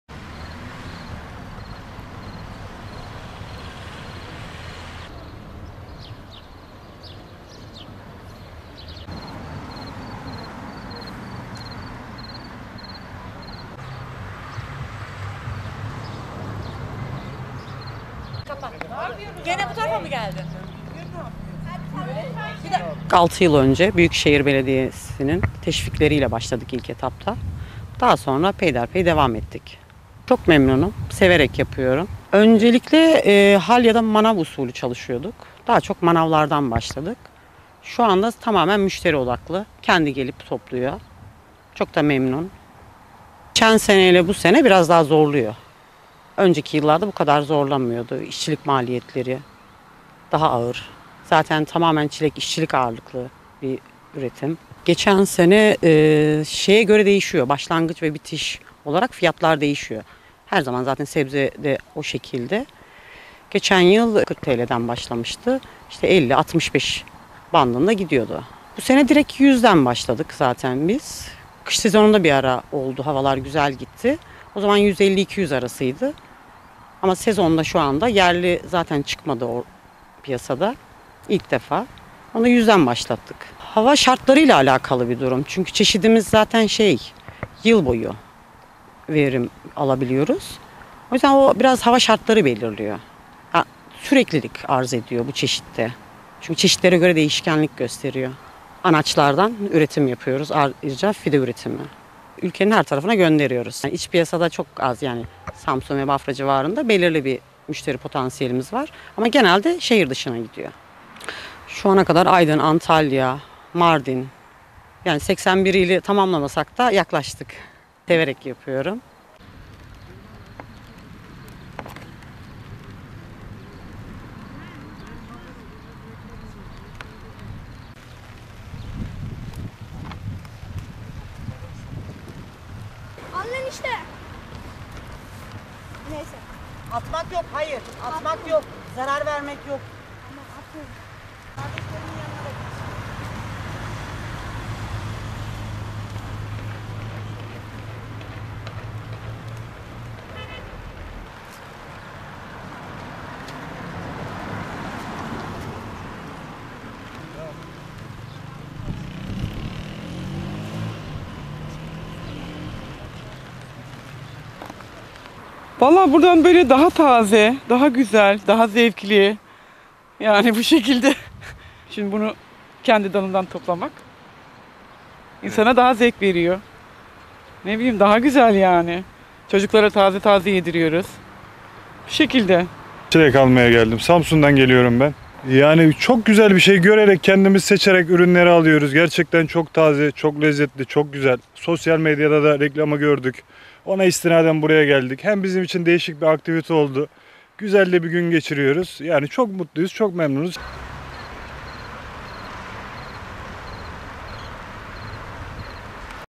Tarladan Taze Hizmet! Müşteriler Alacakları Kadar Kendileri Topluyor
Çilek tarlasına gelen müşteriler, alacakları çilekleri seçerek kendileri topluyor.